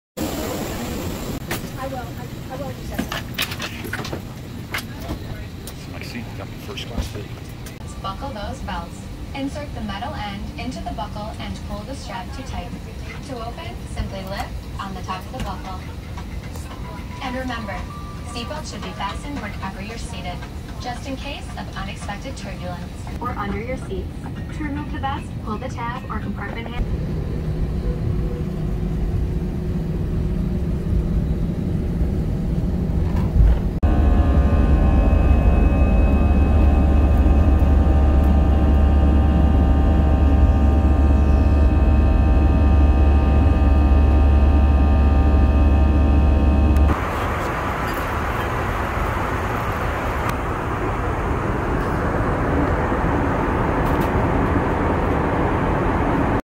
Let's Take a FLIGHT ✈️
Boarding and taking off in a Flight ✈️